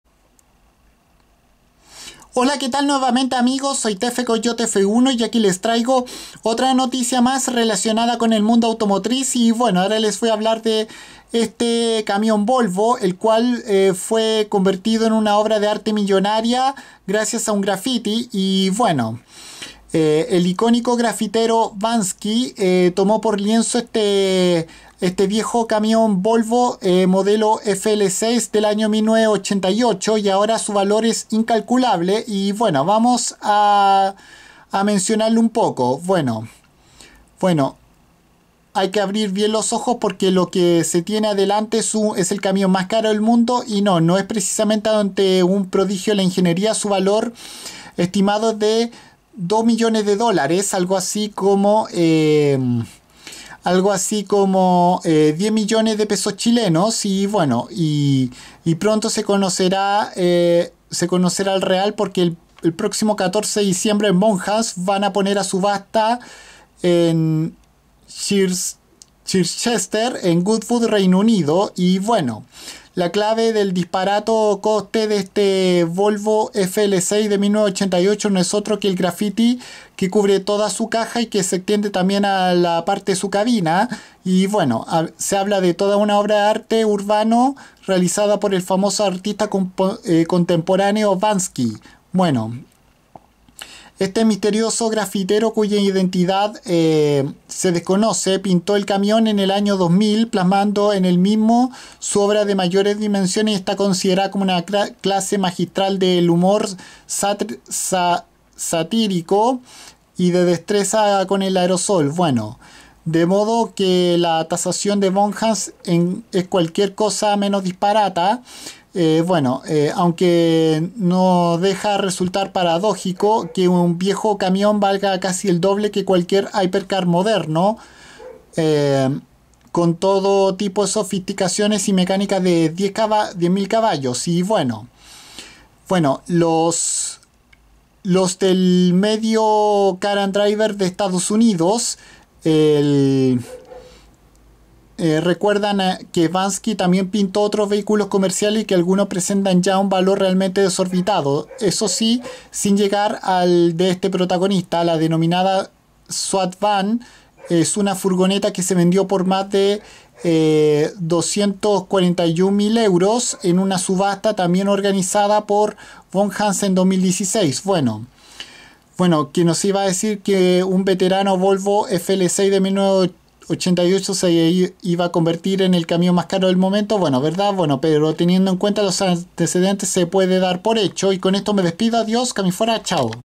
Este viejo camión Volvo FL6 de 1988 fue convertido en una obra de arte millonaria
Un grafitero urbano convirtió a este antiguo camión en una valiosa obra de arte que ahora cuesta muchísimo dinero